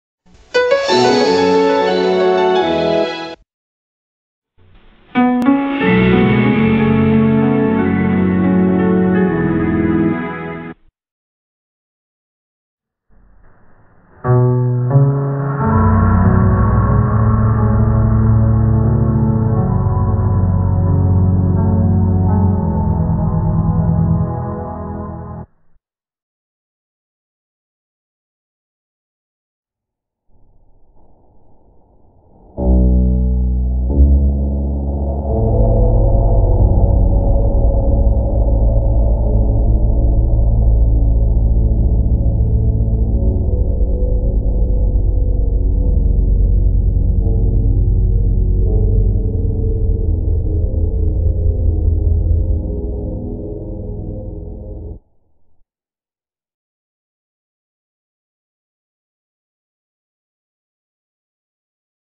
Castle rock entertainment 1989 are slowing down with effects
Title: Castle rock entertainment 1989 are slowing down with effects

List of effects:

1. Normal
2. Threshold with Slow x2
3. Color Hue Cycle with Slow x4
4. Cyan tone with Slow x6